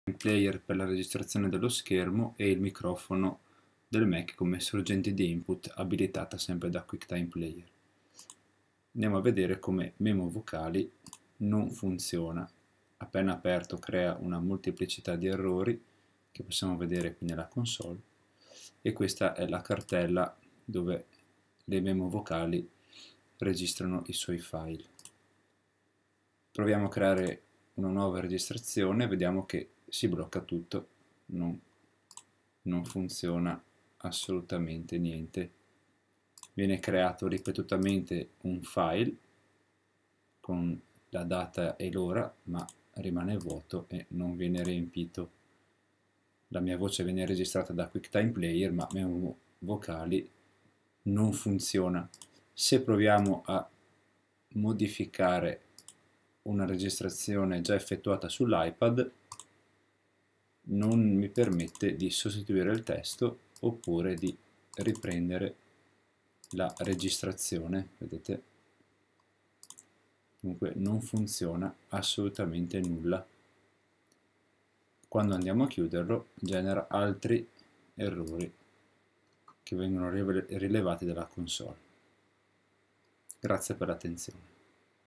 [SOLVED-RISOLTO] Memo Vocali non registra l'audio su MacOS Catalina - Voice memos doesn't rec audio
Problema di registrazione, Memo Vocali ha un bug in MacBook Pro with MacOS Catalina 10.15.3 dove in alcuni casi non effettua la registrazione audio. Di fatto è inutilizzabile. Il problema è stato segnalato ad Apple, qui c'è l'ultima discussione creata sul forum di supporto
L'update 10.15.4 non ha risolto il problema.

Recording problem, Voice Memos has a bug in MacBook Pro with MacOS Catalina 10.15.3 where in some cases it does not record audio. In fact it is unusable. The problem has been reported to Apple, here is the latest discussion created on the support forum